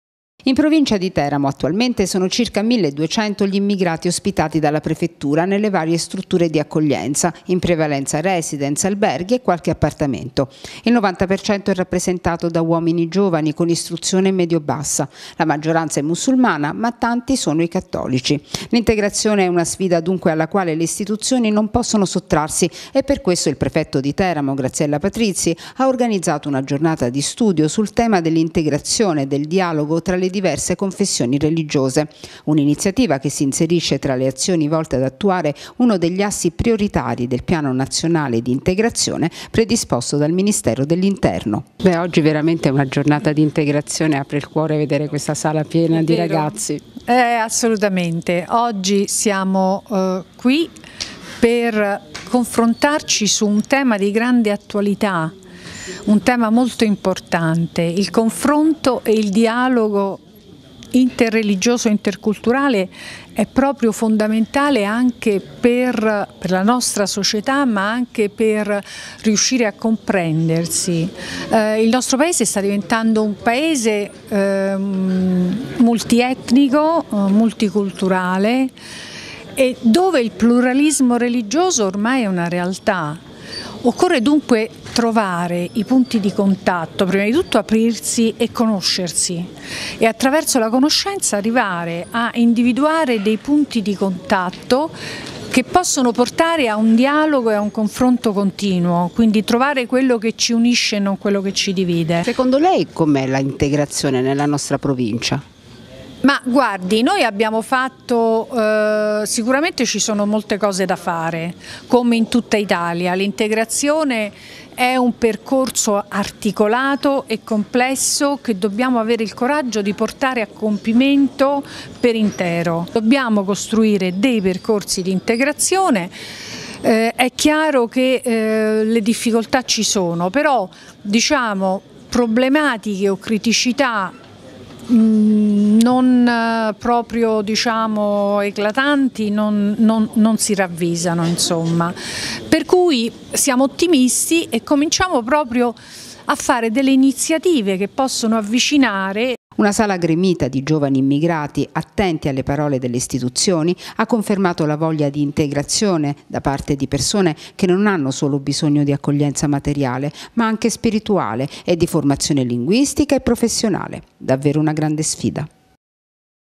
Prefettura: azioni di integrazione e dialogo tra religioni
L'immigrazione in provincia di Teramo non presenta criticità nonostante gli oltre 1000 stranieri attualmente ospitati in centri di accoglienza. Lo sostiene il Prefetto di Teramo Graziella Patrizi che ha organizzato una giornata di incontro per favorire integrazione e dialogo tra diverse religioni